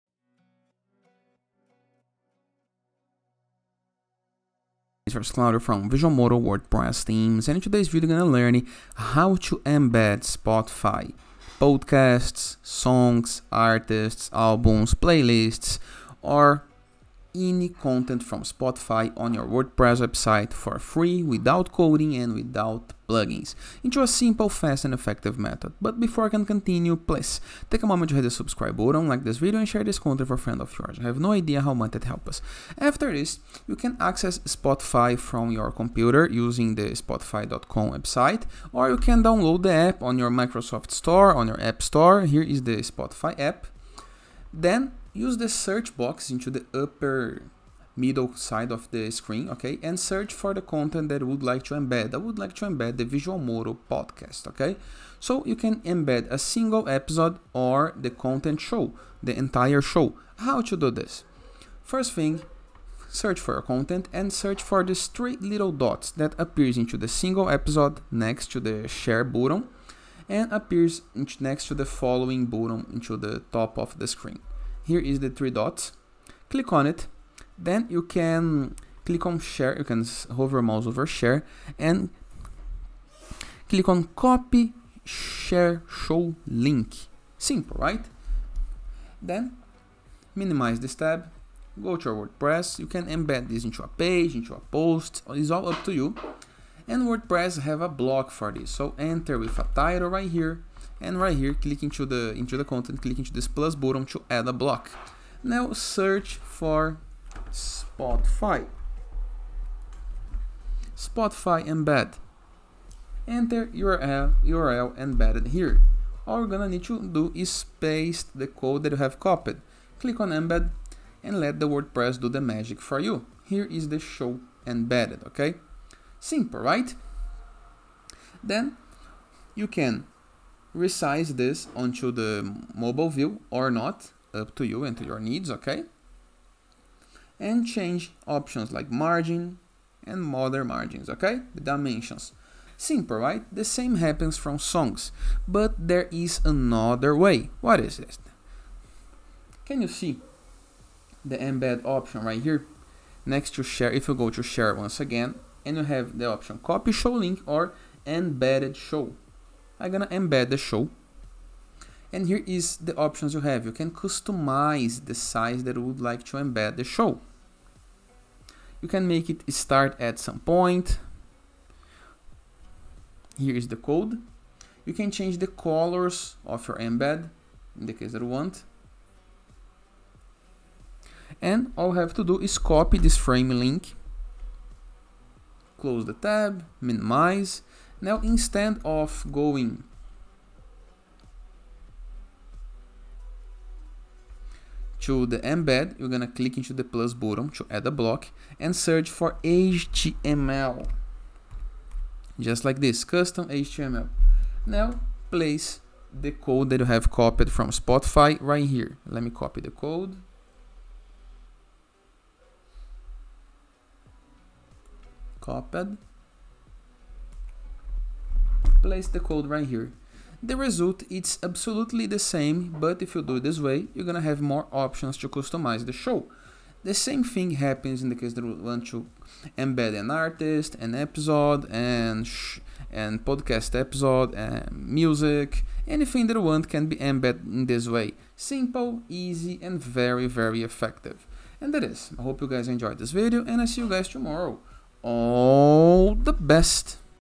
How to Embed Spotify Song, Playlists, Podcasts or Artists in WordPress For Free? 🎧
Want to add Spotify music, playlists, podcasts, or artist profiles directly into your WordPress website — for free? In this quick tutorial, I’ll show you exactly how to embed Spotify content into your posts, pages, or sidebars, making your site more engaging and interactive for your audience!

Perfect for music bloggers, podcasters, artists, creators, and WordPress users who want to add audio content directly from Spotify without using premium tools or complicated plugins.

✅ What You’ll Learn in This Video:
🔹 How to get the Spotify embed code for songs, playlists, podcasts, and artists
🔹 Where and how to paste the embed code in WordPress (Gutenberg + Classic Editor)
🔹 How to use Spotify’s built-in sharing tools
🔹 Tips to customize the size and placement of the player
🔹 Bonus: Best practices to boost engagement with embedded media

🎵 Whether you're showcasing your favorite music, your own podcast, or a curated playlist, this method will work on any WordPress site — fast, free, and responsive!
📢 Found this helpful? Leave a LIKE 👍 and SHARE with other content creators or site owners!
🎧 Watch now and learn how to seamlessly embed any Spotify content into your WordPress site — for free and with no hassle! 🎶✅📲